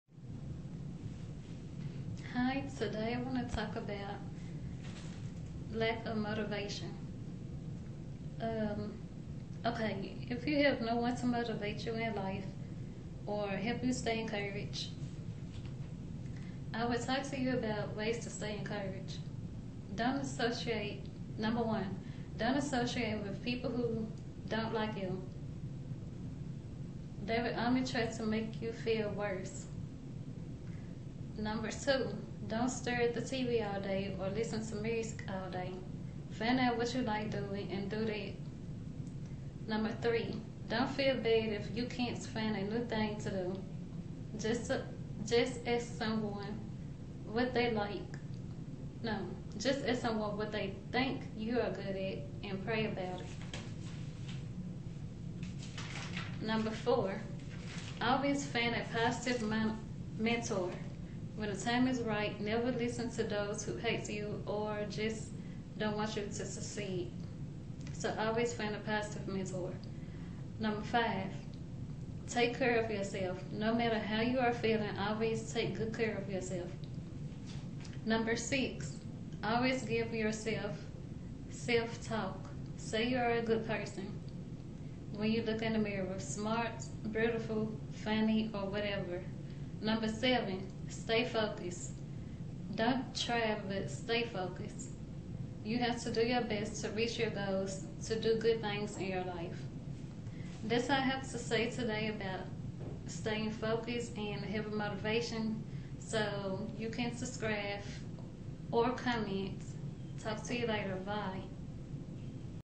Lack of motivation to do better?🙋🙌🏃
I am talking about how to motivate yourself to do good things in life.I hope you learn something from this video.Have a nice day.😊